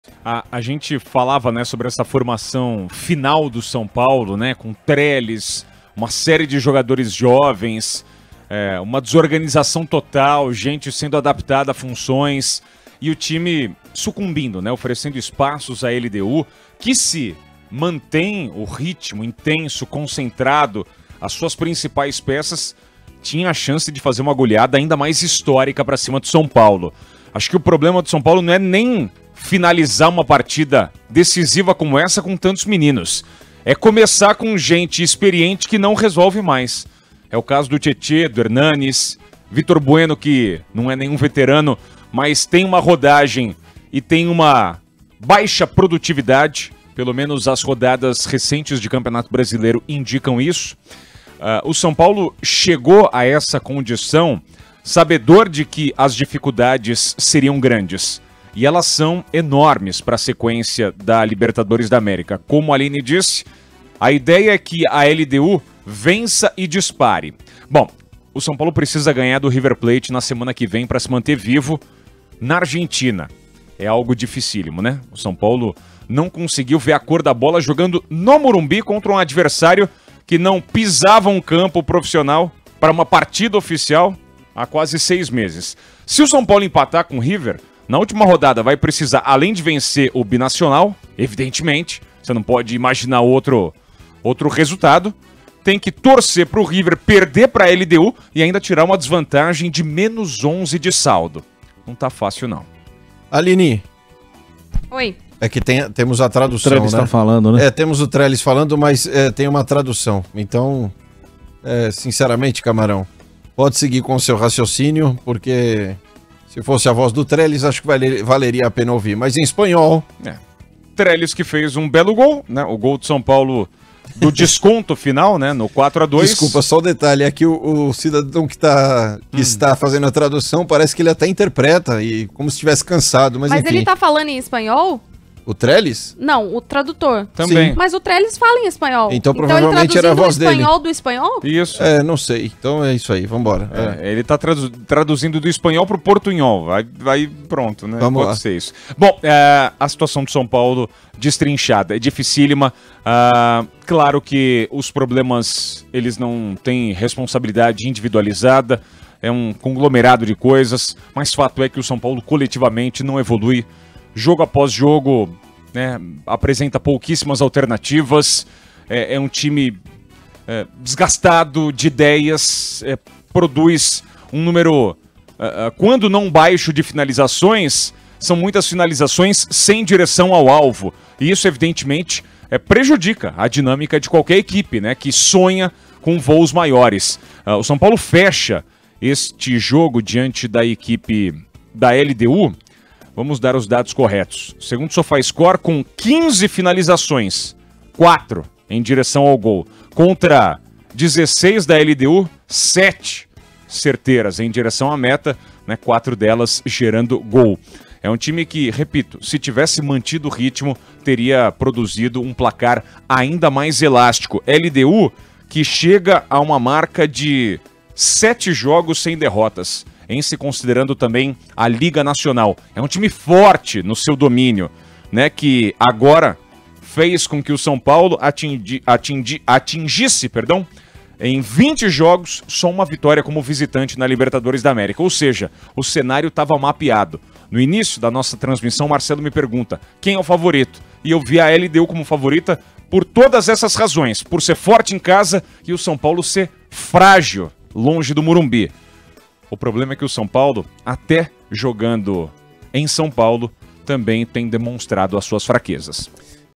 Bruno Camarão analisa vitória da LDU sobre o São Paulo
A LDU vence o São Paulo na quarta rodada da Libertadores. Confira a análise de Bruno Camarão (@brunocamarao).